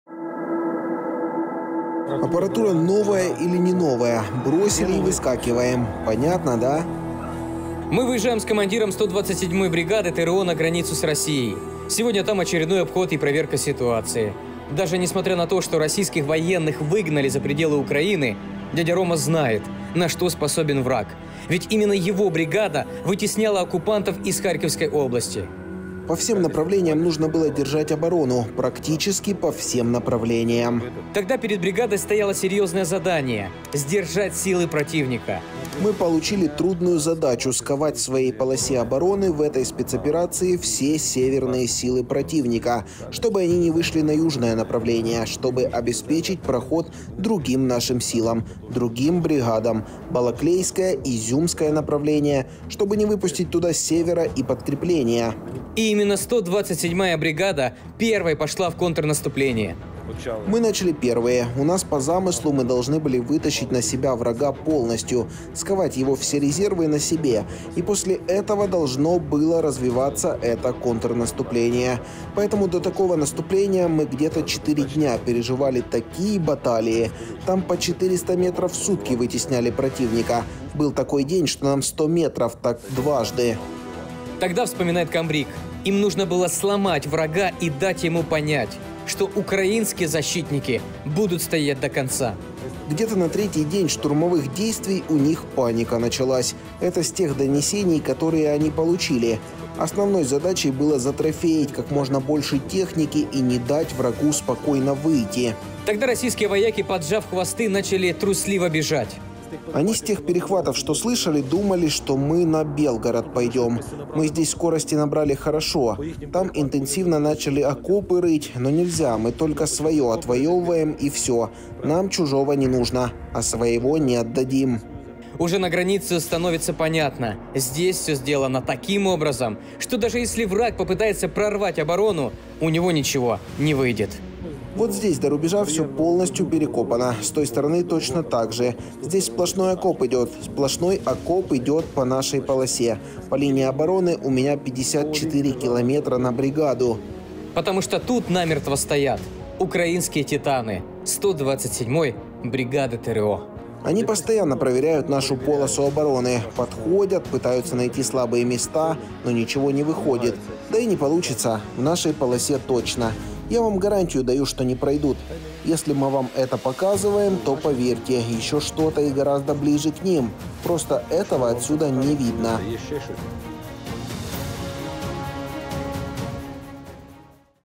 💪 Они драпали и боялись, что мы пойдем на Белгород! — Дядя Рома, комбриг 127 бригады ТРО
В нашем сюжете мы покажем комбрига 127 бригады ТРО, чья бригада воевала и воюет в Харьковской области. Они были одними из тех героев, кто прогонял врага с Украинской территории. Подробнее - в сюжете.